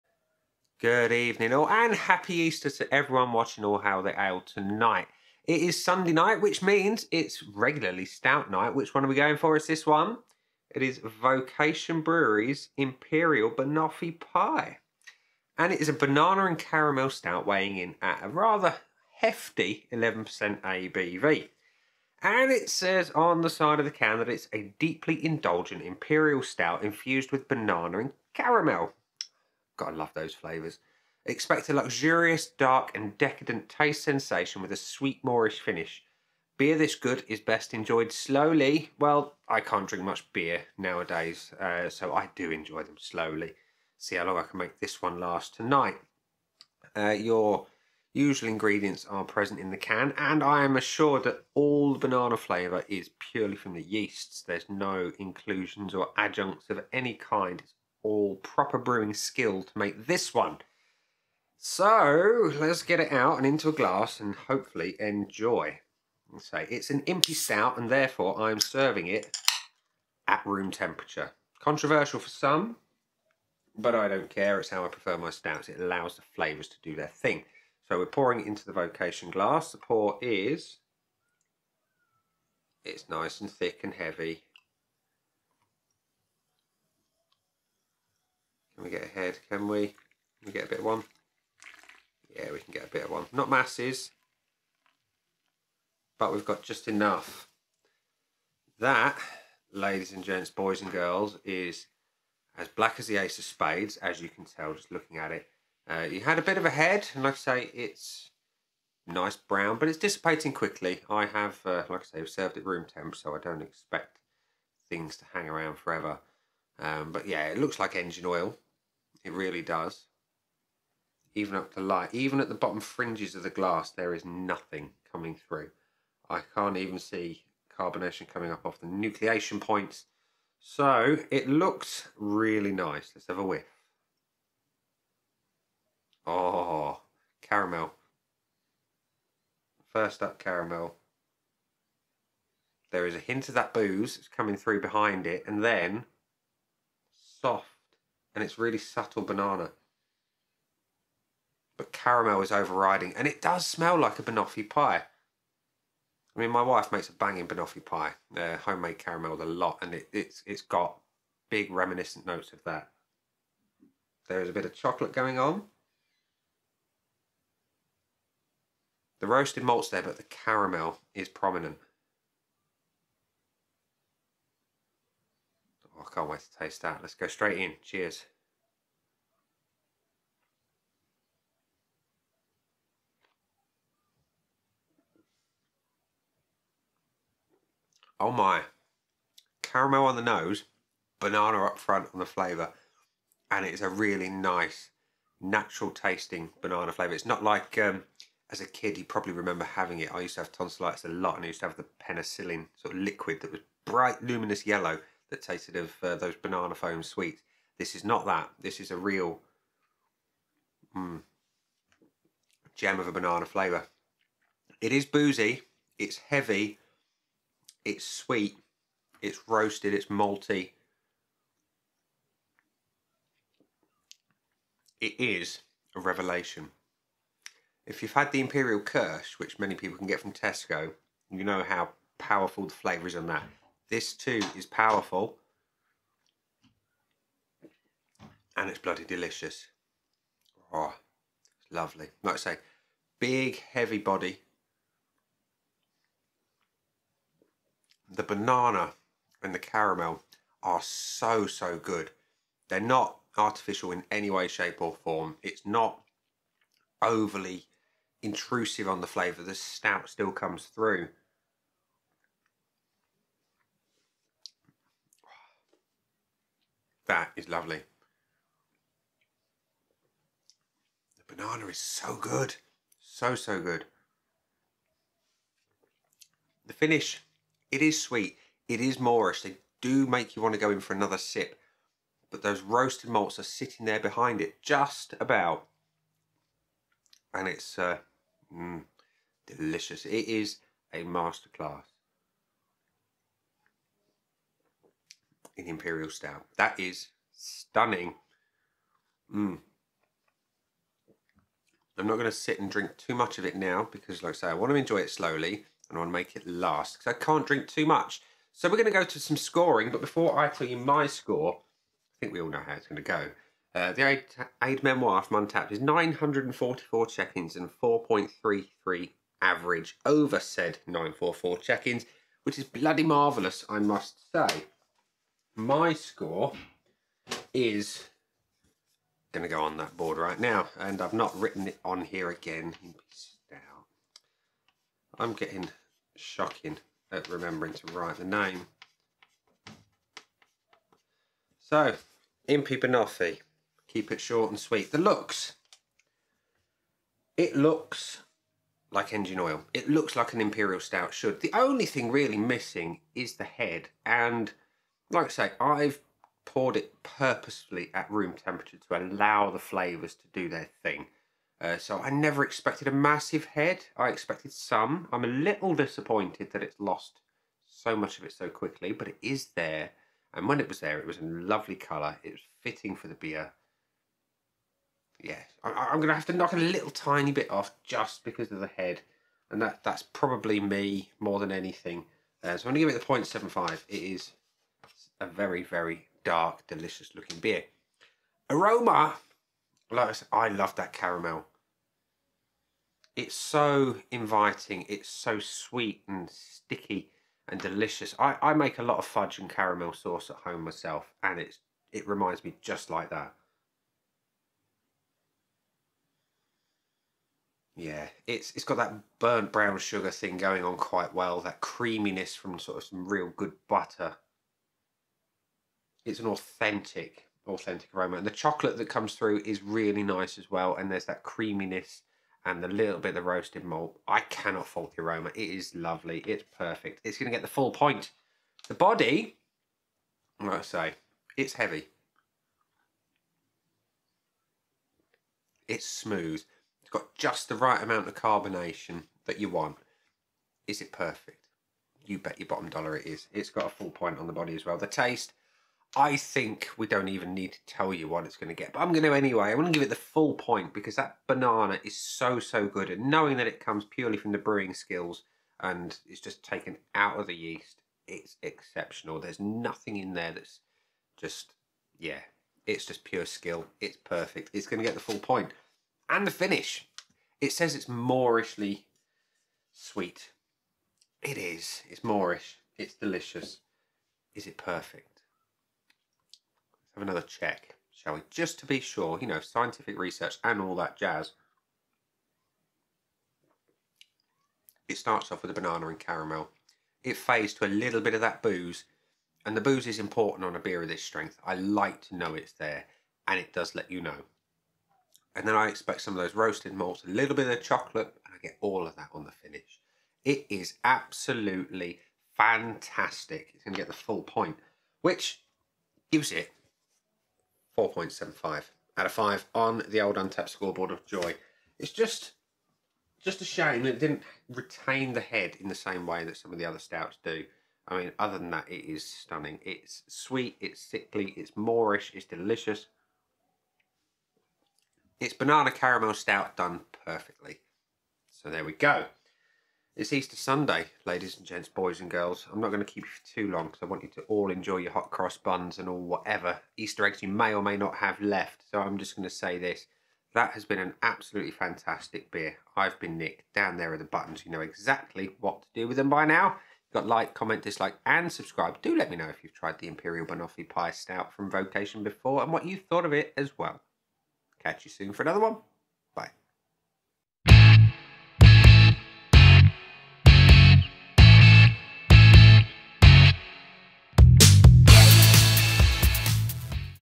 VOCATION BREWERY - Imperial Banoffee Pie Stout Review
Vocation Brewery bring another powerful imperial stout to the party, this time in a banana caramel guise.  Will it be an Easter revelation, or a disappointing drab affair?

Brewery: Vocation Brewery
Beer: Imperial Banoffee Pie Stout
ABV: 11%